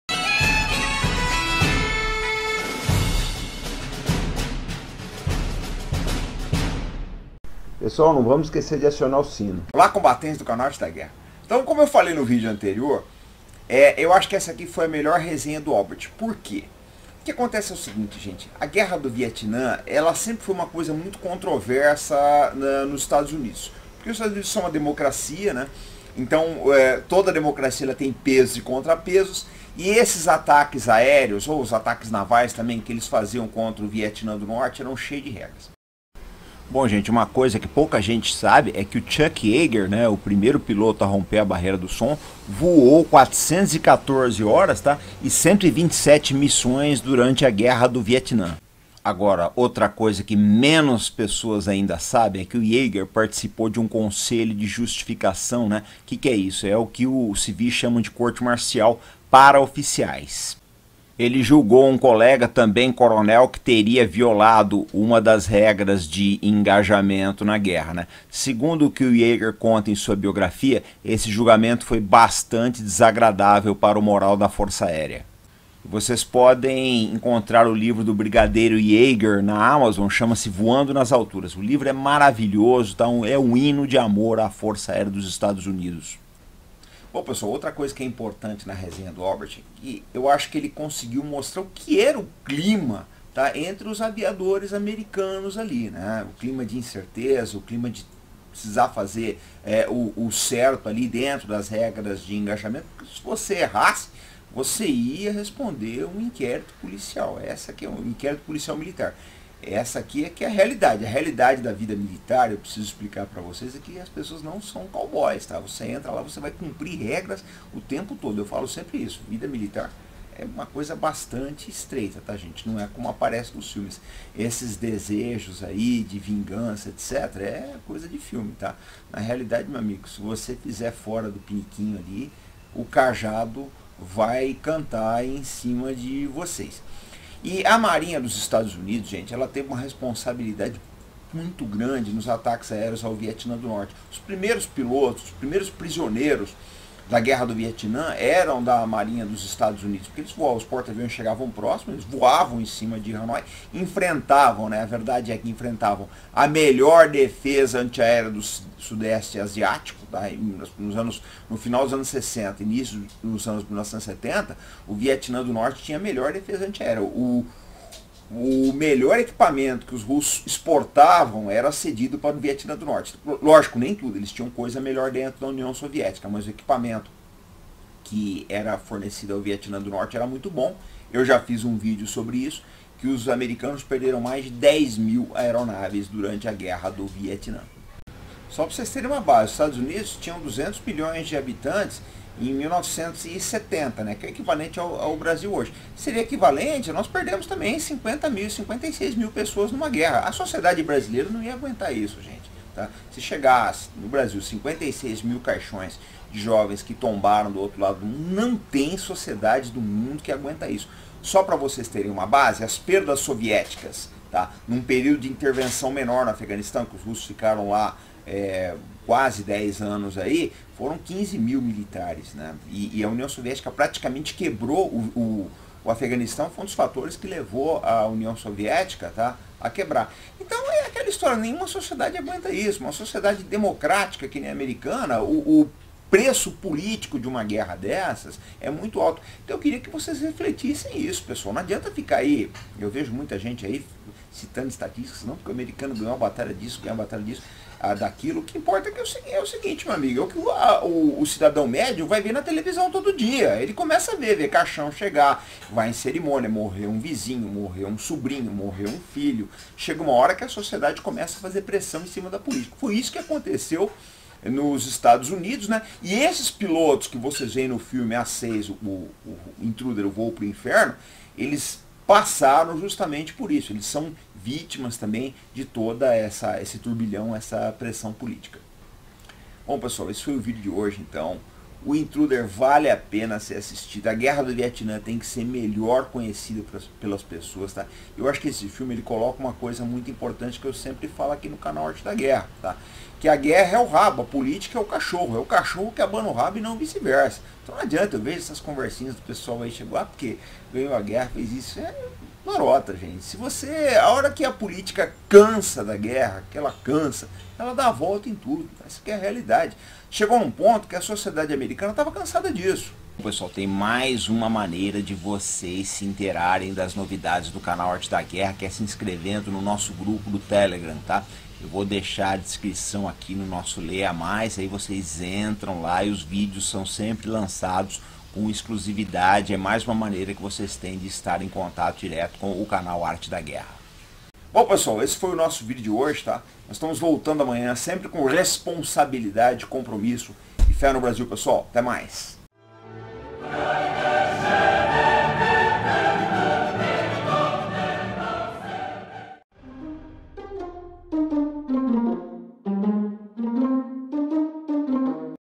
ANÁLISE : INTRUDER: UM VOO PARA O INFERNO-VÍDEO 1075
Saiba também qual foi a participação de Chuck Yeager na Guerra do Vietnã.